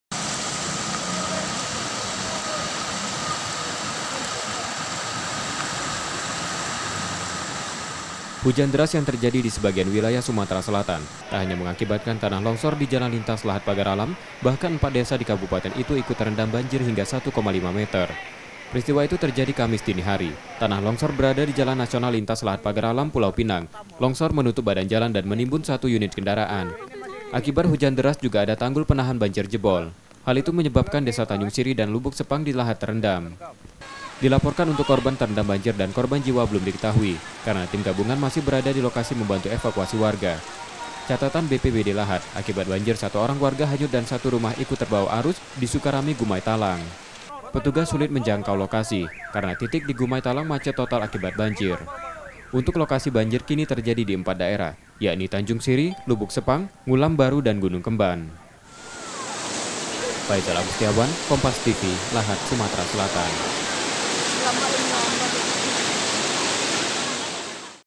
Banjir Bandang Dan Tanah Longsor Kembali Terjang Lahat
Banjir Dan Tanah Longsor Kembali Menimpa, Kabupaten Lahat Sumatera Selatan. Sedikitnya Ada Enam Desa Yang Rumah Warganya Terendam Air, Sementara Jalan Penghubung Kabupaten Tertutup Longsor.